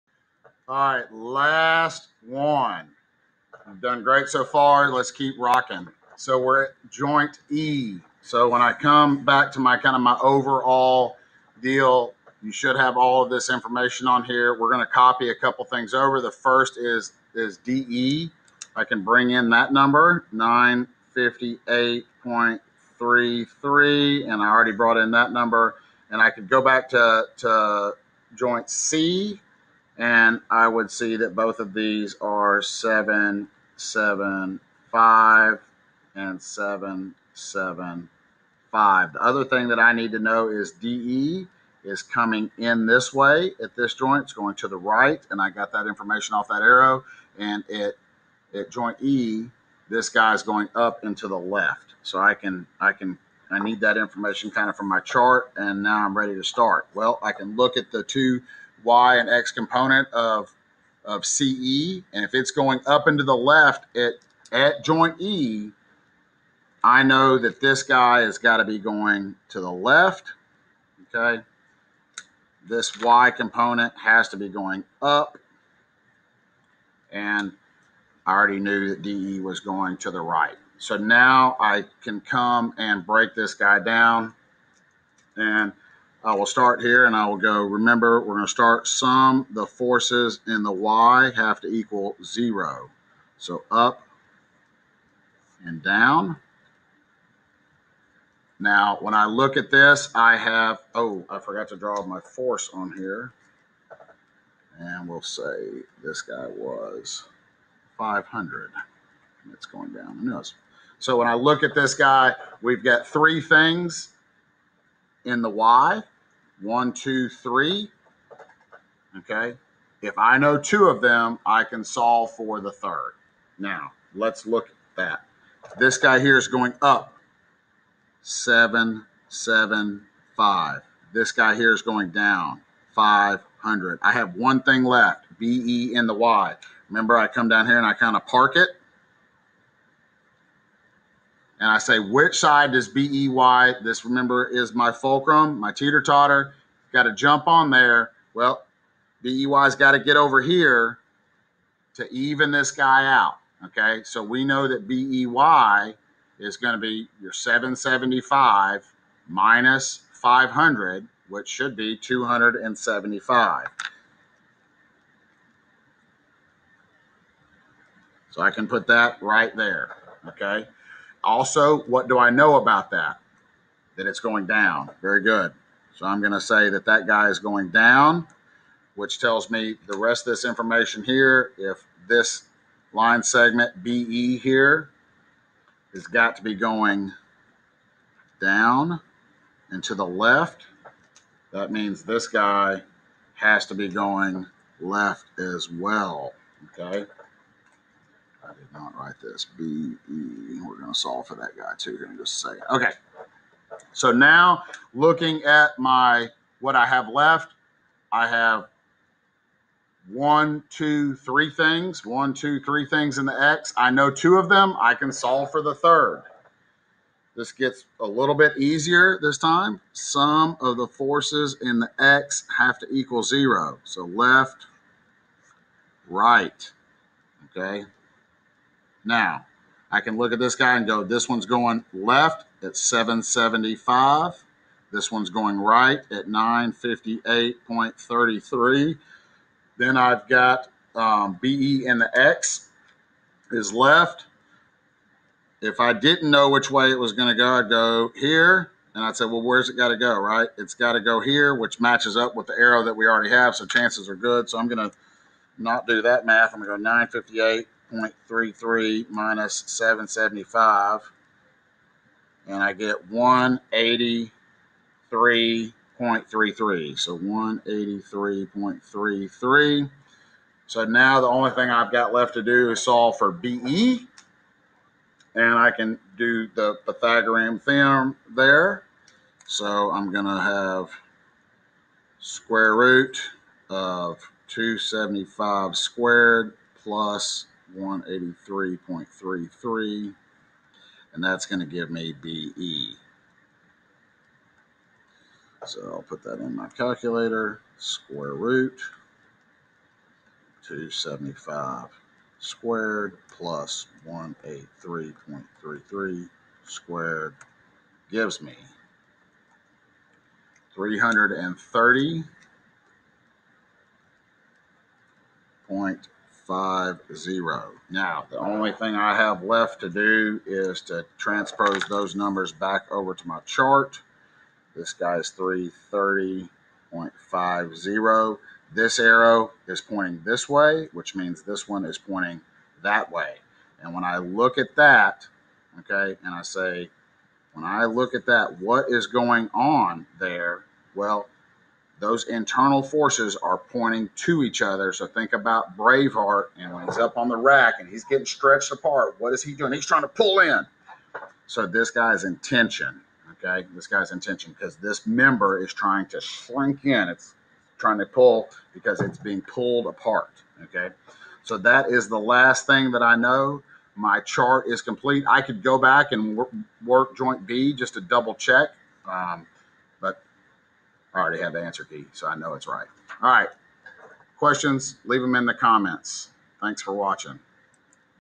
How to solve a Truss joint E(Truss Video 6)
My videos are another way to solve the truss Mr.Lockwood solves in his wonderful videos. If you want a more mathematical way to do it, please check his videos out